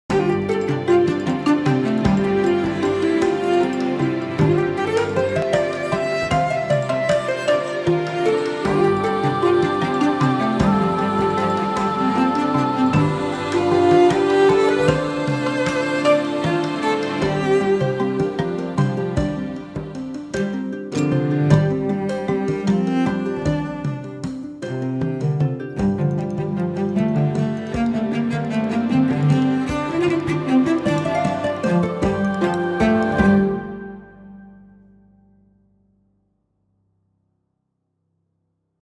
Alma Pulveris - Animatic Sketch
Hello, here's a WIP for the intro video animatic. u w u Hope you like it (I know not everything is understandable, yet).
The song was commissioned from Raphaël Hinojosa.